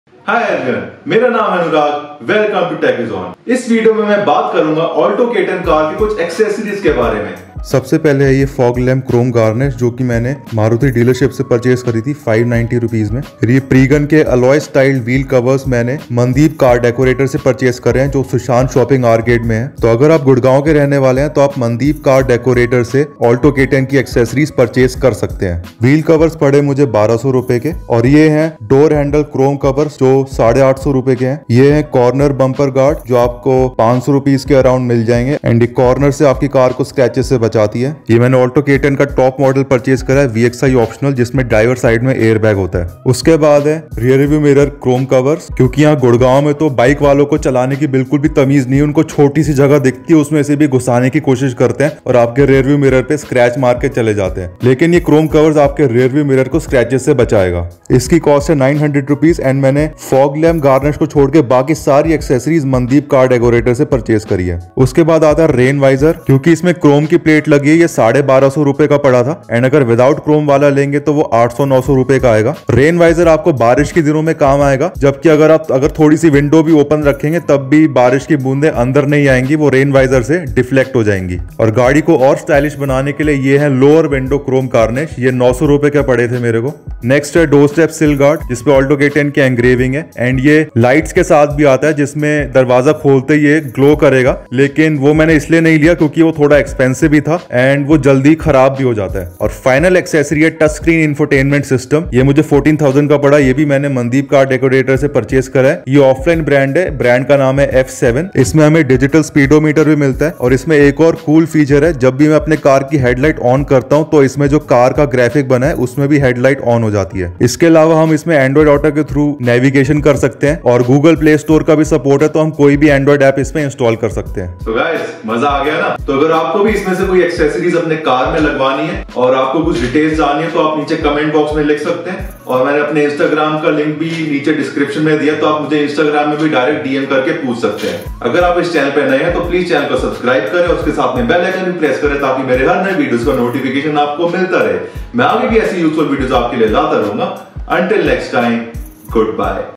Alto K10 Car Accessories With Prices
I talked about the car accessories I have purchased for my Alto K10 Vxi(o).

Fog Lamp Chrome Garnish is purchased from dealership @590/-
Corner Bumper Guard, Rearview Mirror Chrome Cover, Rain Visor, Lower Window Chrome Garnish, Door Handle Chrome Catch Cover, Door Step Sill Guard, Touchscreen Infotainment System are purchased from Mandeep Car Decorator in Sushant Shopping Arcade, Gurgaon